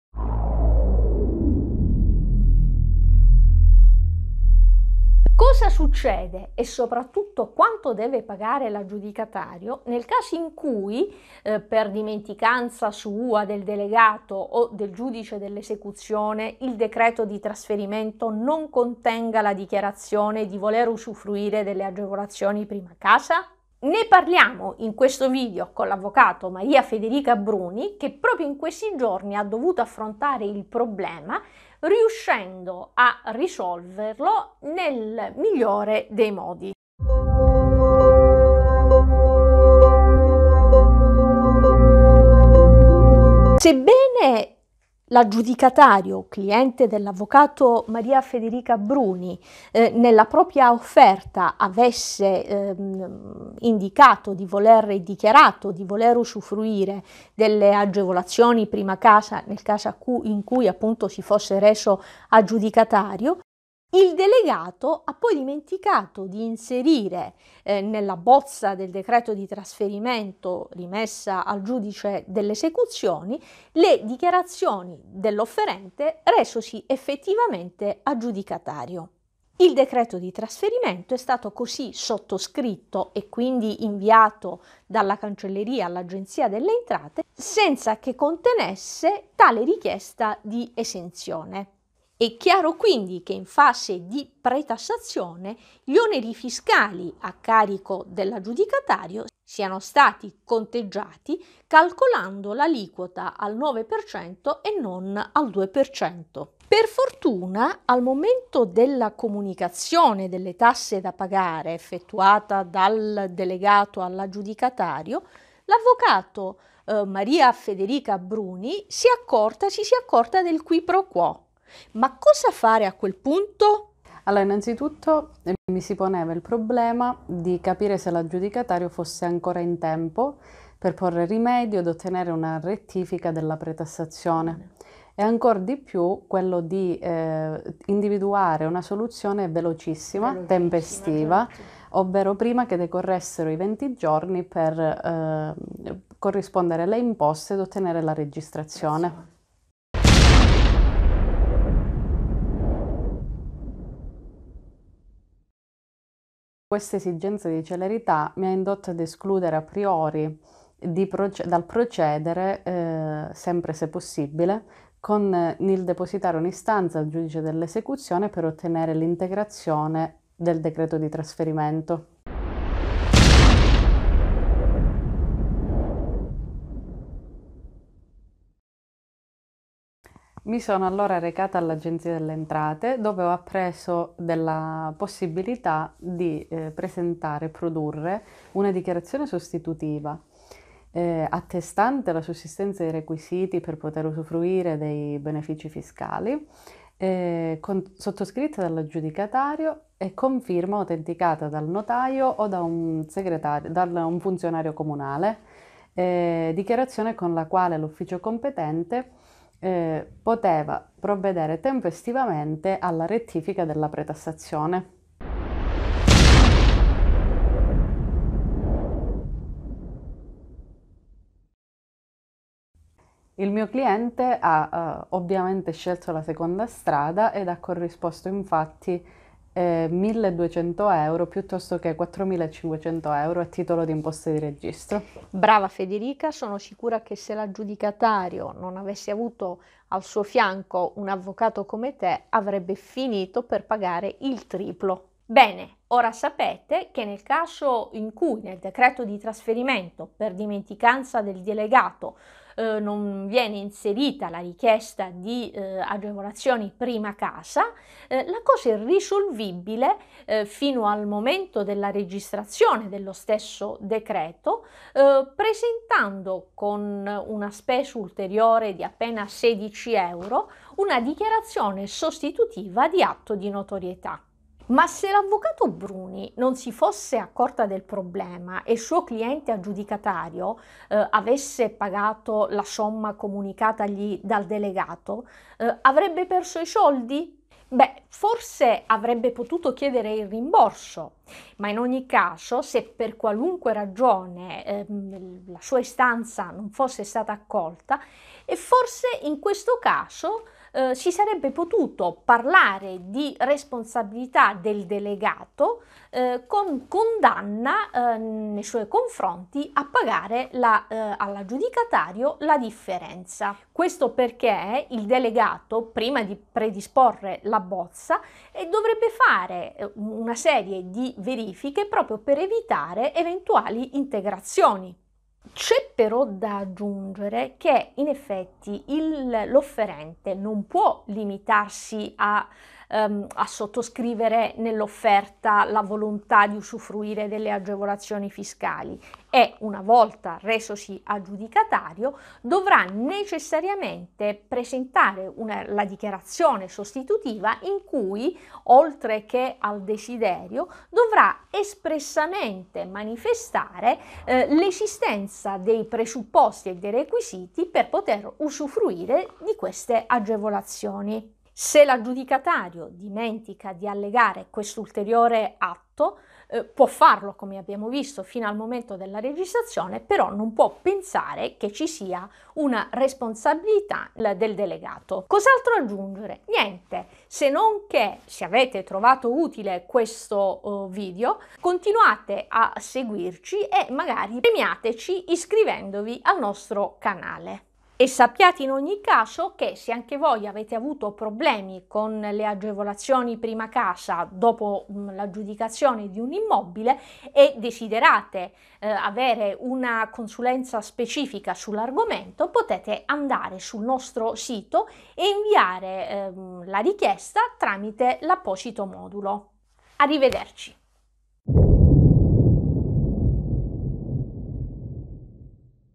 Come comprare PRIMA CASA all'asta
Le agevolazioni prima casa nella vendita all'asta
- Cosa succede, e soprattutto quanto deve pagare l’aggiudicatario, nel caso in cui, per dimenticanza sua, del delegato o del giudice, il decreto di trasferimento non contenga la dichiarazione di poter usufruire delle agevolazioni prima casa?
Ne parliamo in questo video con l’avv. Federica Bruni che proprio in questi giorni ha dovuto affrontare il problema, riuscendo per fortuna a risolverlo nel migliore dei modi.
Se guardate il video fino alla fine saprete anche fino a quando è possibile presentare la dichiarazione di esenzione prima casa.